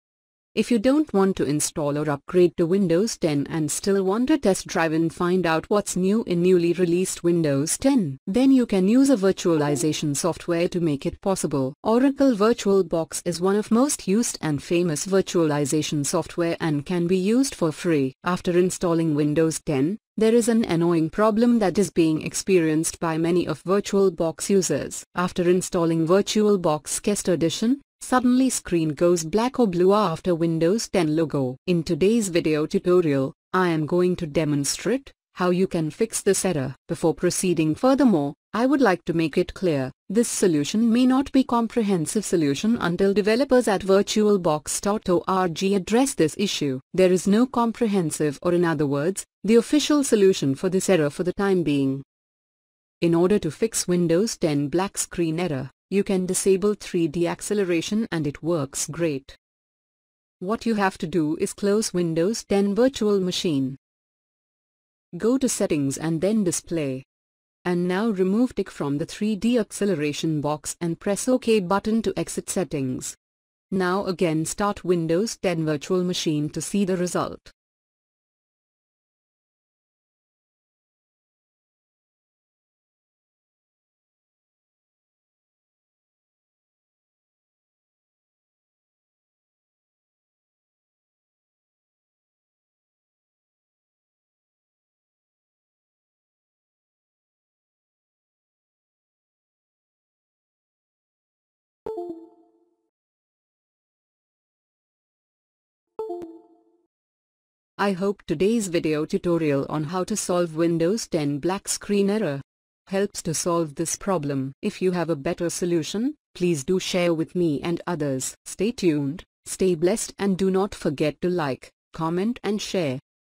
How To Fix Windows 10 Black Screen In VirtualBox?
This video tutorial is on How to fix Windows 10 black screen in Virtualbox after installing guest addition?

After installing Windows 10, There is an annoying problem that is being experienced by many of VirtualBox users. After installing Virtualbox Guest addition, Suddenly Screen goes black or blue after Windows 10 logo.

In this Windows 10 black screen help tutorial, I am going to demonstrate, How you can fix this error? Before proceeding furthermore, I would like to make it clear, This solution may not be comprehensive solution until developers at virtualbox.org address this issue. There is no comprehensive or in other words, the official solution for this error for the time being.

How To Fix Windows 10 Black Screen of Death Error In VirtualBox?

In this How To Fix Windows 10 Black Screen In VirtualBox? You can easily fix Windows 10 black screen of death error at your own.

How To Fix Windows 10 Black Screen After installing Virtualbox Guest addition In VirtualBox?

In order to fix Windows 10 black screen error, You can disable 3D acceleration and it works great To Fix Windows 10 Black Screen After installing Virtualbox Guest addition In VirtualBox.